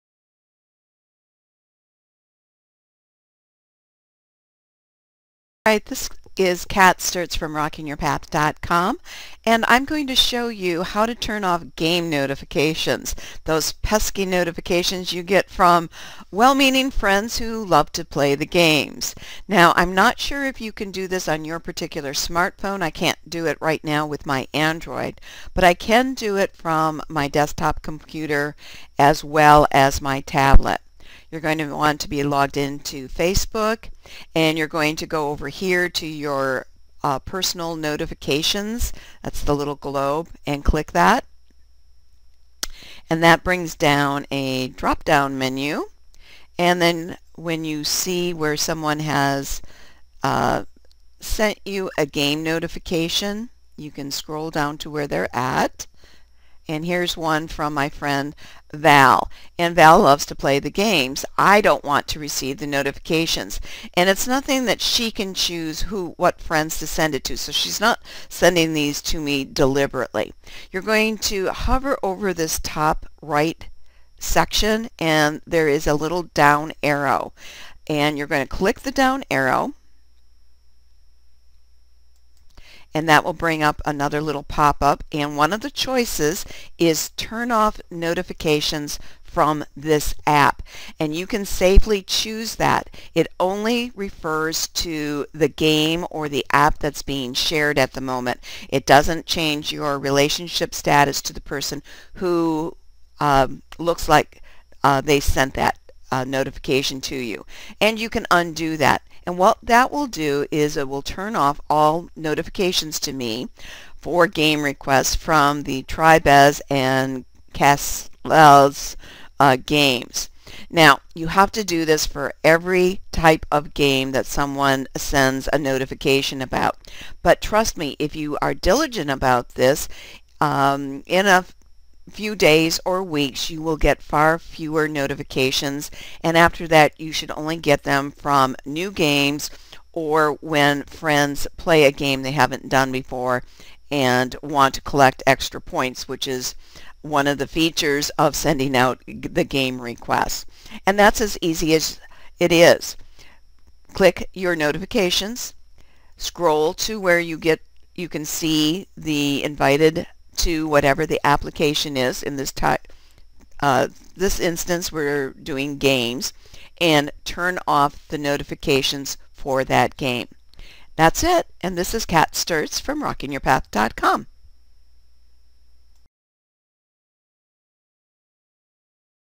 Fast Action Tips: Turn off Facebook game requests
Learn an easy way to reduce all those pesky game notifications you receive on Facebook.

Kat Sturtz
Business Life & Marketing Coach